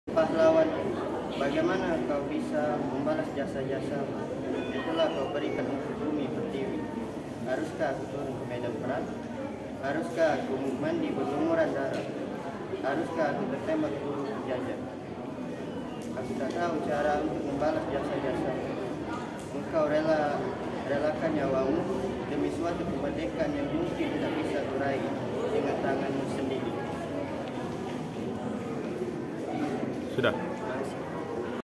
081 AHMAD FANCA - SMKN 3 PLG - PAHLAWANKU
Pahlawanku
      Oleh : Reza Hidayat

Pahlawanku..
Bagaimana ku bisa
Membalas jasa-jasamu
Yang telah kau berikan untuk bumi pertiwi

Haruskah aku turun ke medan perang
Haruskah aku mandi berlumuran darah
Haruskah aku tertembak peluru penjajah
Aku tak tahu cara untuk membalas jasamu

Engkau relakan nyawamu
Demi suatu kemerdekaan yang mungkin
Tak bisa kau raih dengan tanganmu sendiri
Pahlawanku.. engkaulah bunga bangsa